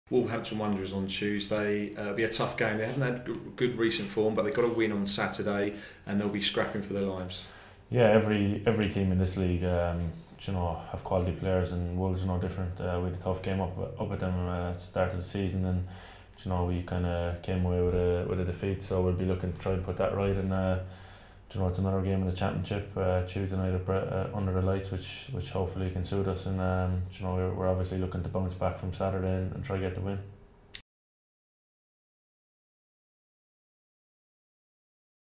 John Egan's Pre-Match Interview ahead of Wolverhampton Wanderers
Full interview on Bees Player